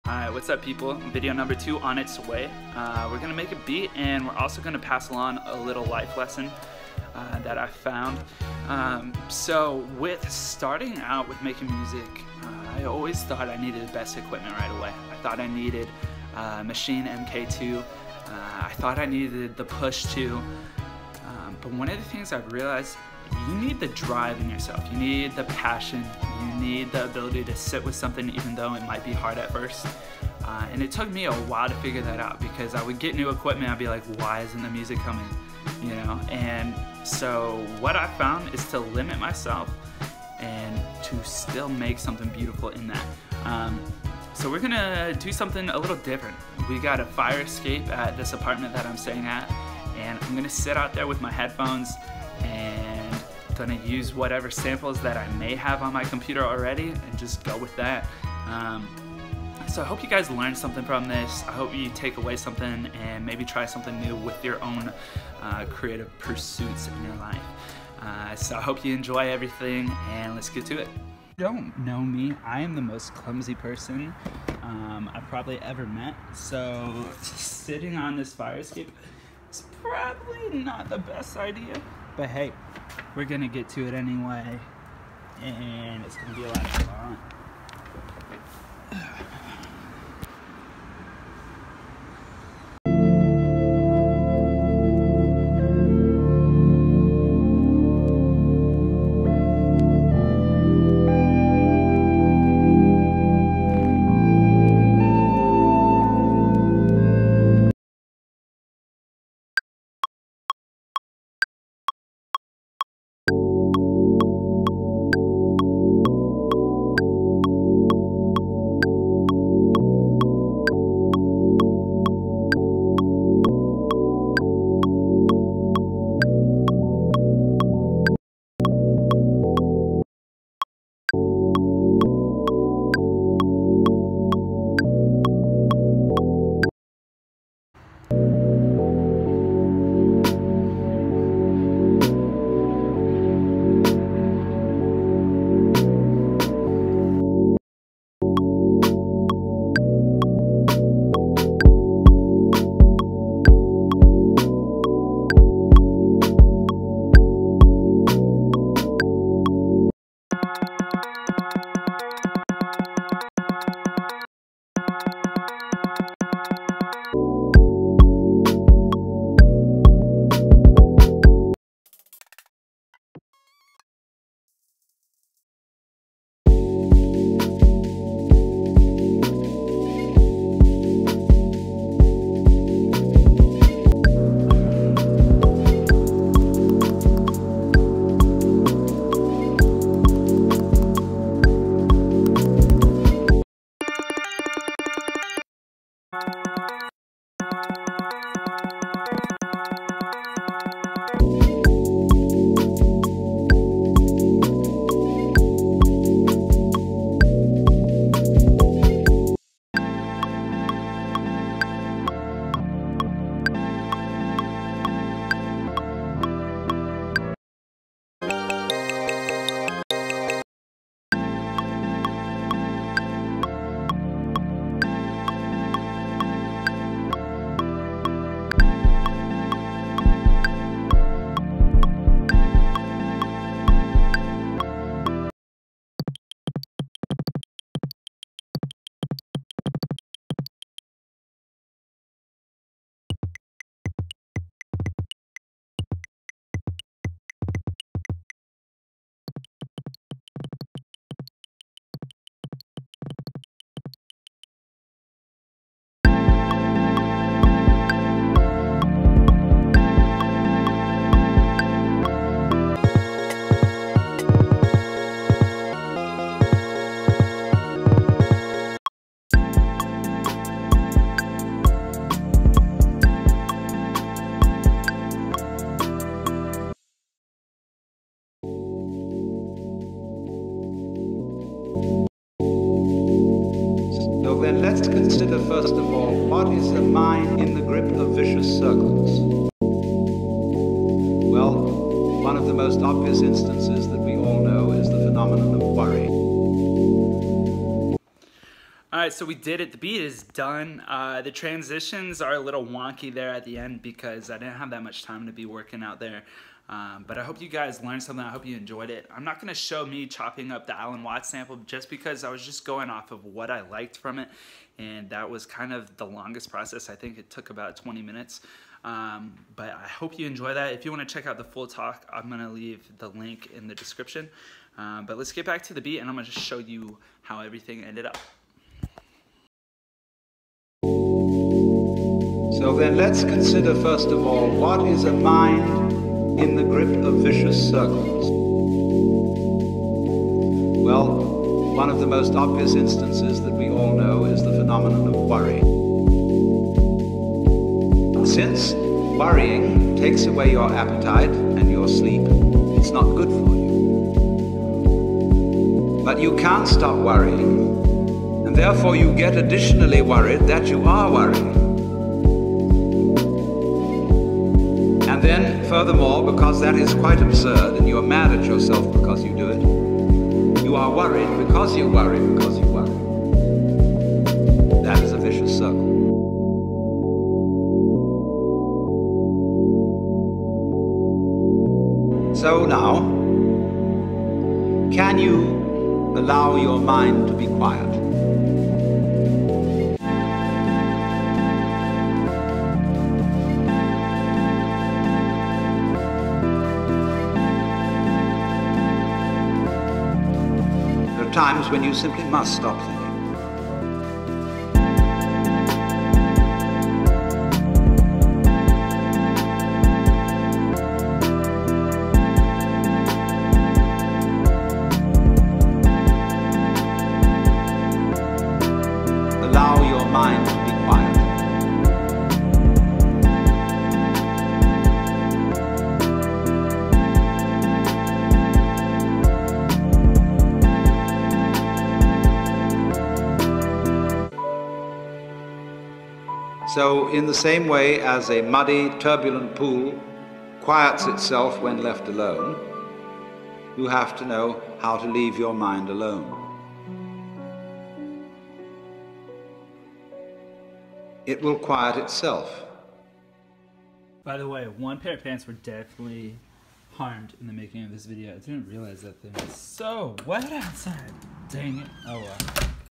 Making a beat on a fire escape!
Took a trip up to Chicago last week and realized that even if I didn't have all of my equipment, it still wasn't enough to stop me from trying to create something!
In this video I take you through making a beat from scratch with limited resources!
With only one charge of the battery and whatever samples that were already downloaded on my computer, I sat down on that fire escape with limits that were meant to be pushed!
Once you can see what is possible with just a keyboard, you realize there is no excuse to get something done!
Even if you are traveling, can't afford anything, not paying attention in class!
No matter what, you can find that energy inside of you if you eliminate the excuse and simply create!
Hopefully this will inspire at least one of you to do something you didn't think you could!